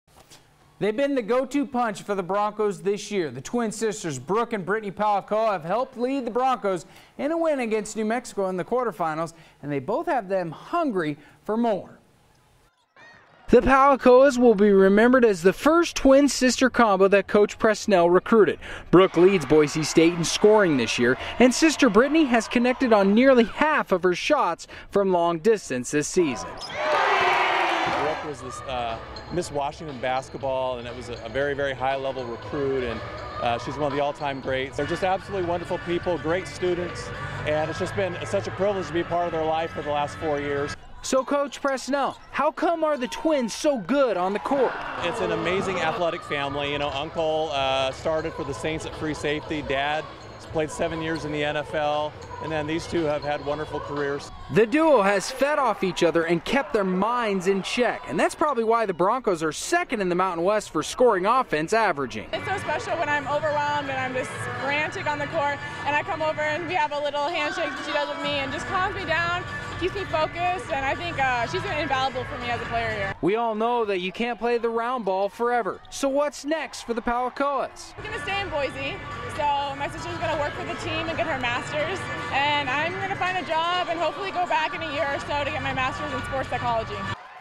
Pahukoa Sisters are the difference maker for BSU
The Twin Sisters Brooke and Brittney Pahukoa have helped lead the Broncos in a win against New Mexico in the quarterfinals of the Mountain West Tournament as both of them are hungry for more.